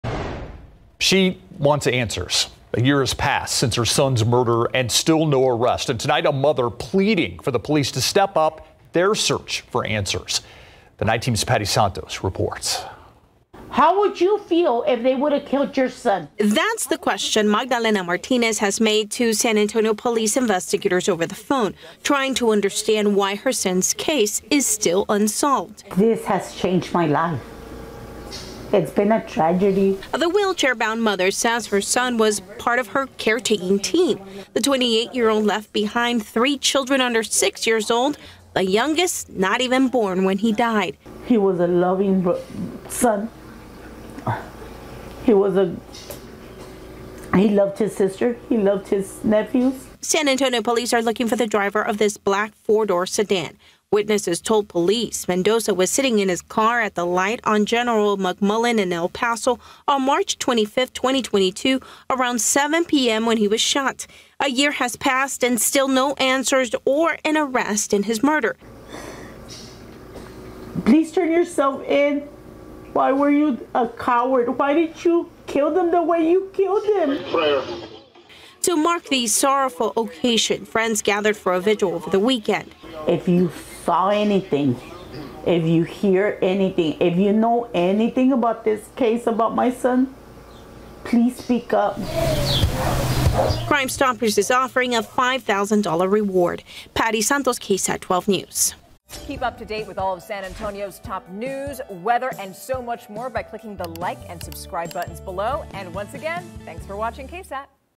Mother of murdered man still waiting for an arrest more than 1 year after son’s death
Juan Mendoza, 28, was killed while in his car waiting at a light on General McMullen and El Paso on March 25, 2022.  His mother says there's still no arrest made in the case.